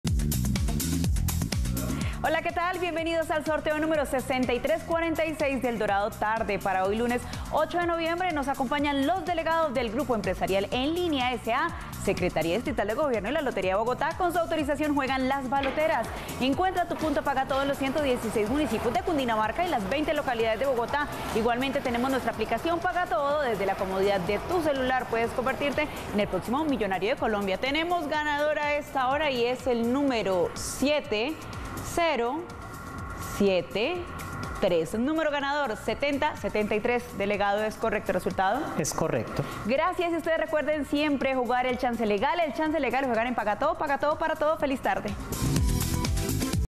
Resultado EL DORADO TARDE del lunes 08 de noviembre de 2021 ✅🥇🔥💰
Hola buenas tardes, te presentamos los resultados de las loterías y chances en Colombia, vamos con el Resultados del Chance Dorado tarde del lunes 08 de noviembre de 2021.